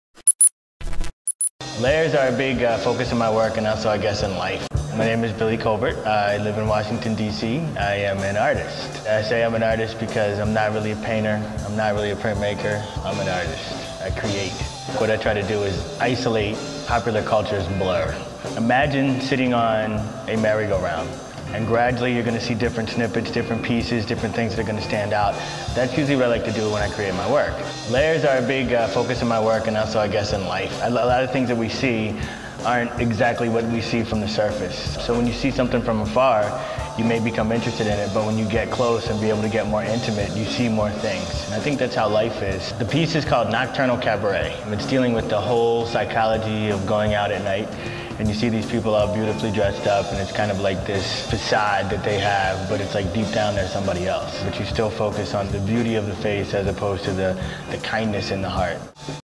My First Project - Medium.m4v
Take a peek at this informative Currents video on Morton Fine Art's artist Billy Colbert as he discusses the inspiration for his amazing mixed media works.